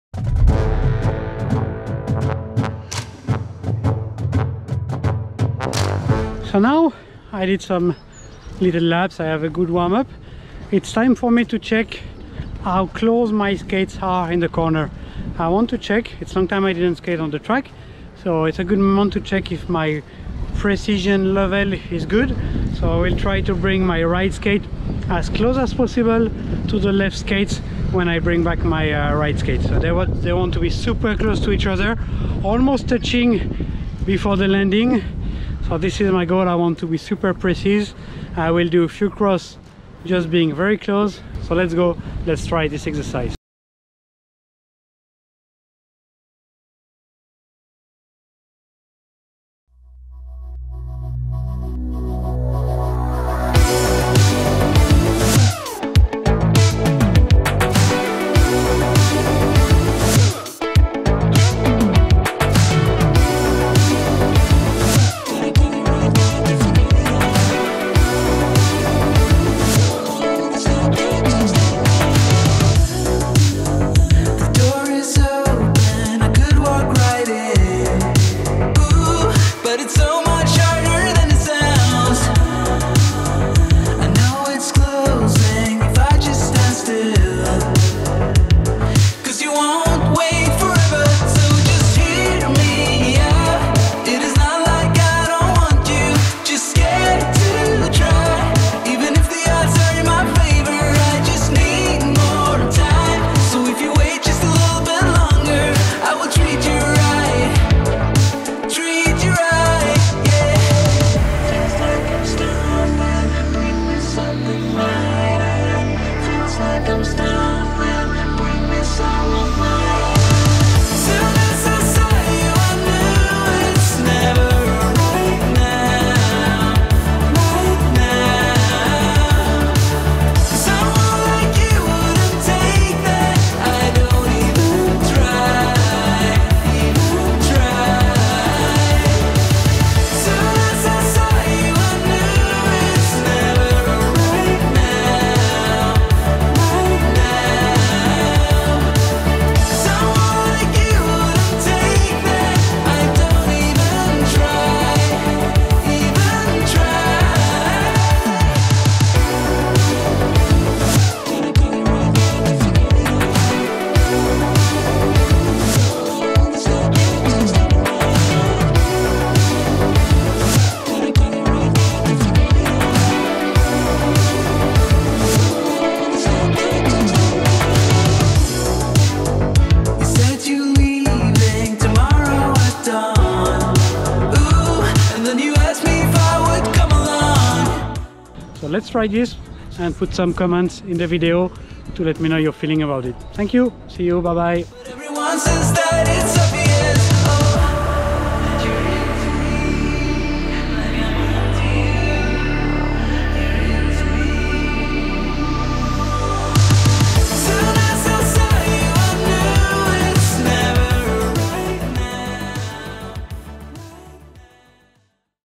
focus on precision during crossover technique (pascal briand vlog 349)
here is a little session in which i focus on the precision during my crossover on inline speed skates.